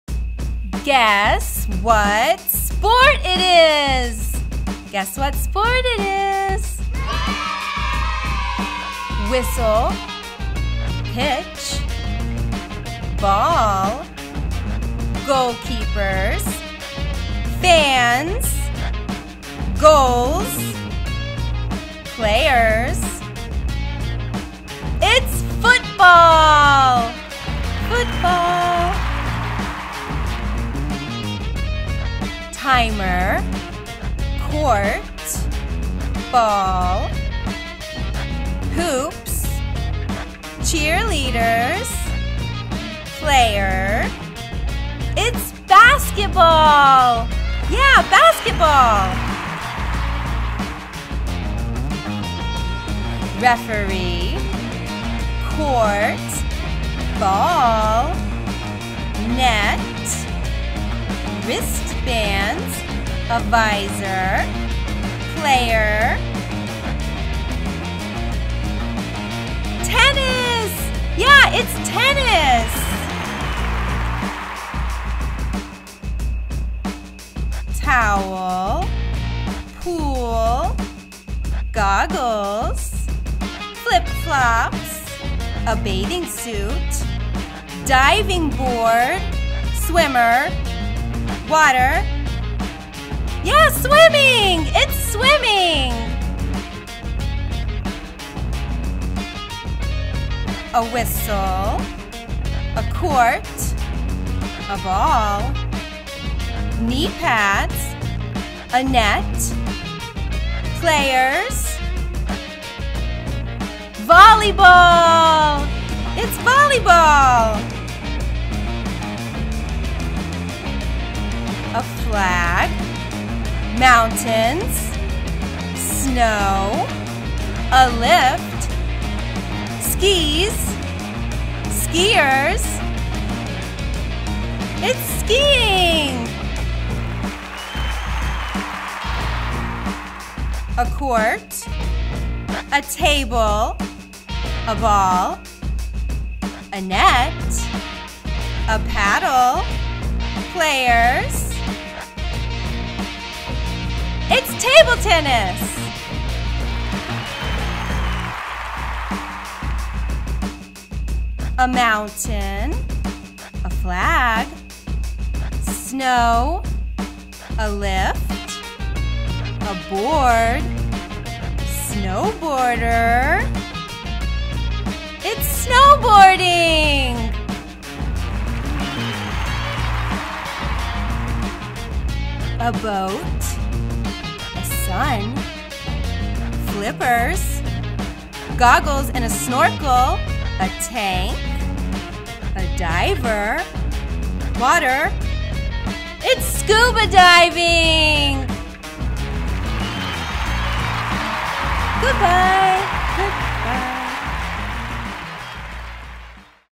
Guess what sport it is!
Let's learn the vocabulary about sports with this guessing video. Voiced by Amanda Ozyurek.